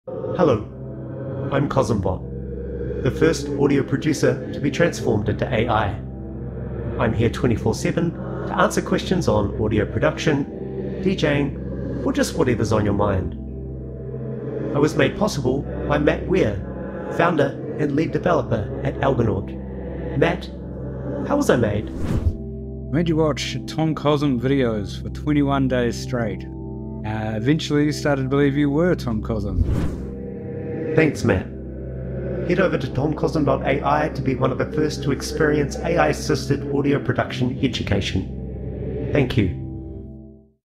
Introducing the AI that thinks it's Tom Cosm - Ask it Anything...
An AI bot trained on the music production knowledge of Tom Cosm, made by Matt Algonaut

We made an AI watch 21 days of Tom Cosm videos, until it became convinced it was actually Tom Cosm. Now you can ask it questions about all the music production knowledge Tom has shared over the years.

AI Video Art by Visual Overdose @zacflanagan